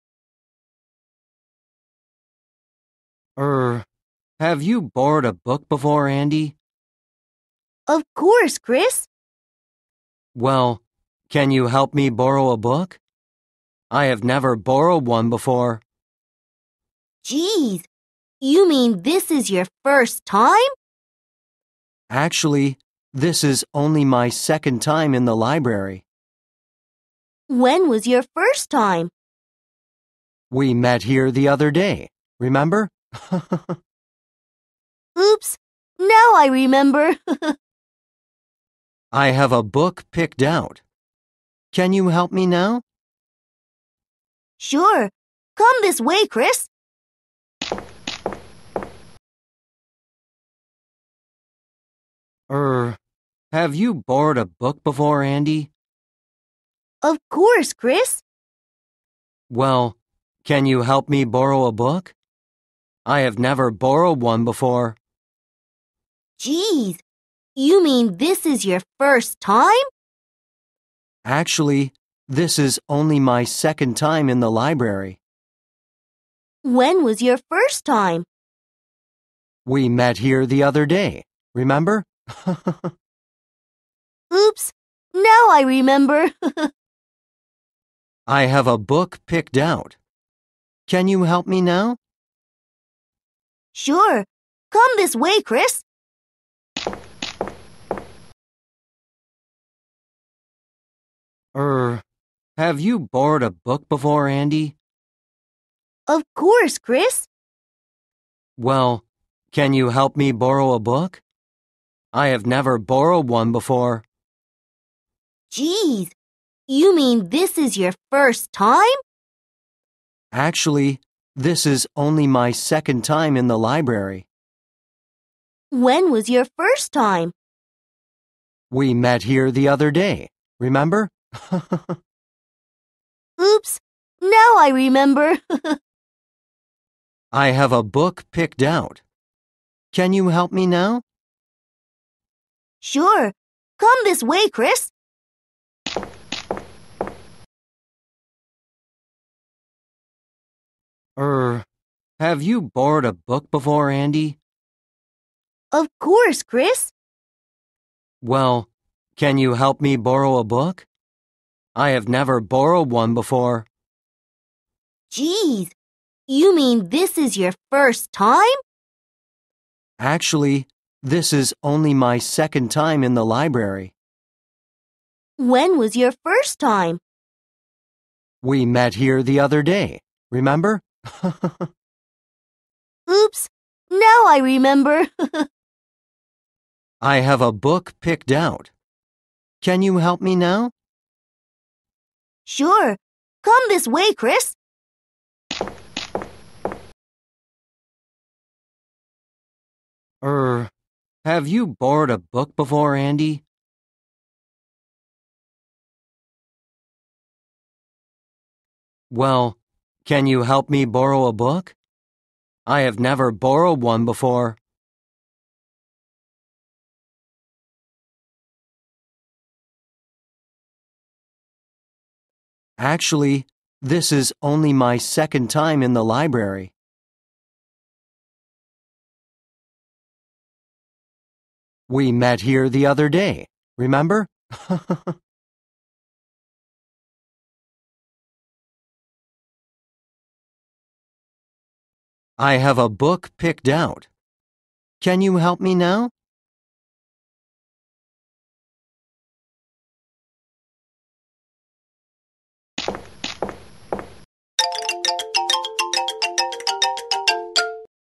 [반복반복] 영어회화116 - 현재시제와 동사구를 이용한 문장 학습 심화3편
2번 보고 듣고, 영어 스크랩트 없이 1번 또 보고 듣고, 모든 스크립트 없이 1번 더 보고 듣고, 마지막은 주인공 역할로 말하기(롤 플레잉)
(Er) Have you borrowed a book before, Andy? 이전에 책을 빌려본 적이 있니, 앤디?
Of course, Chris. 물론이죠, 크리스.
(Well) Can you help me borrow a book? 책을 빌리는 것 좀 도와주겠니?
I have never borrowed one before. 난 이전에는 빌려본 적이 없어서.
(Geez) You mean this is your first time? 이번이 처음이라는 뜻인가요?
Actually, this is only my second time in the library. 실은, 도서관에 온 것도 두 번밖에 안돼.
When was your first time? 처음은 언제였는데요?
We met here the other day, remember? (haha) 우리 지난 번에 이곳에서 만났었잖니, 기억나니?
(Oops) Now I remember. (giggles) 이제 기억나요.
I have a book picked out. 책을 골랐어.
Can you help me now? 이제 좀 도와주겠니?
Sure. Come this way, Chris. 물론이죠. 이쪽으로 오세요, 크리스.